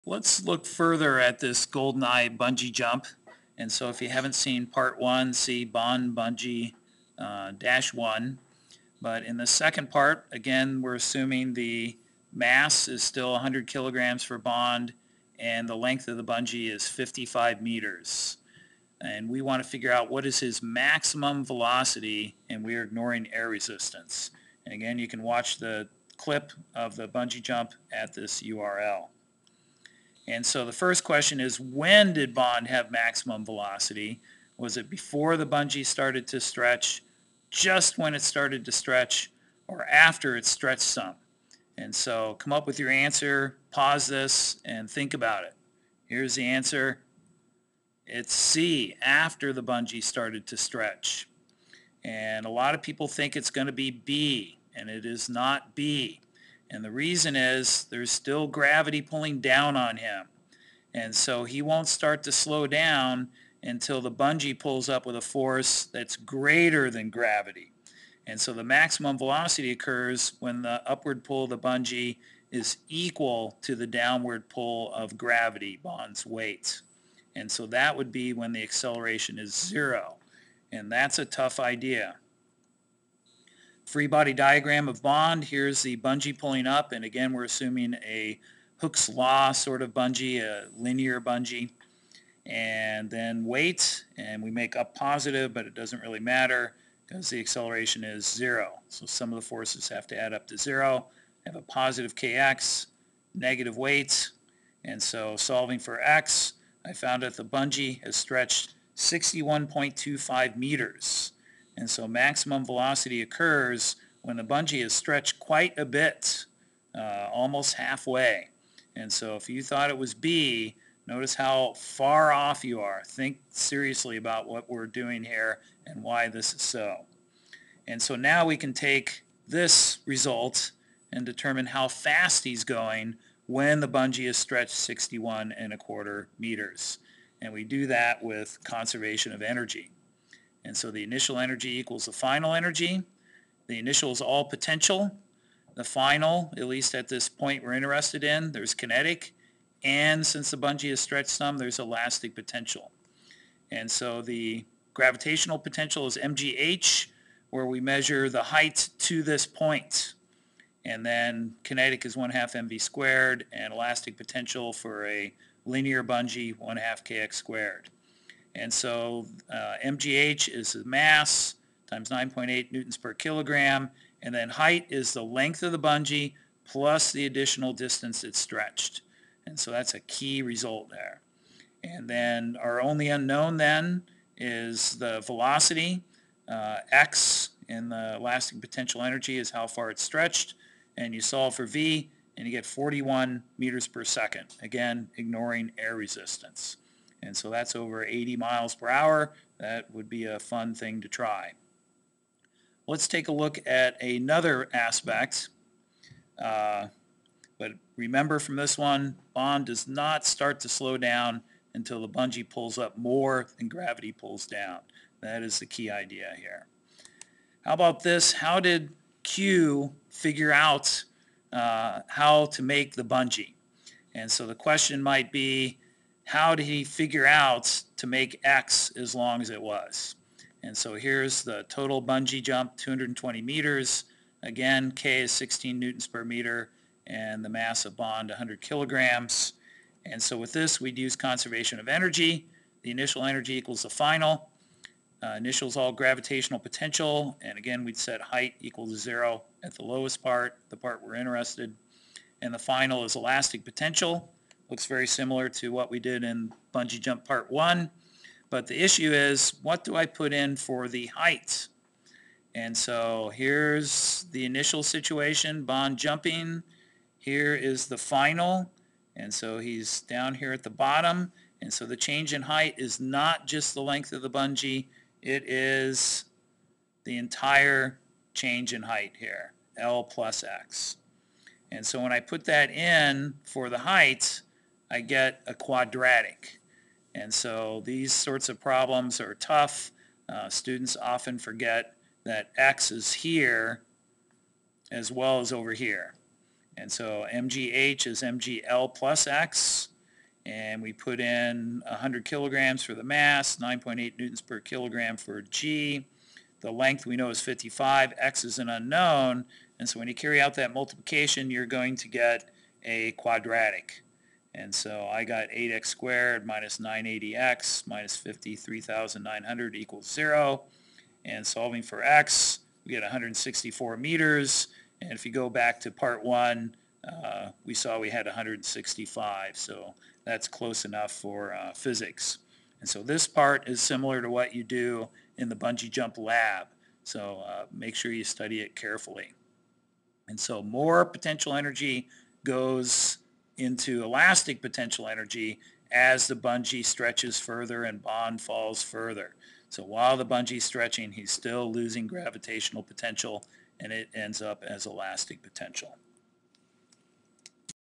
BondBungee-2.mov
Part 2 of using physics to analyze the bungee jump scene.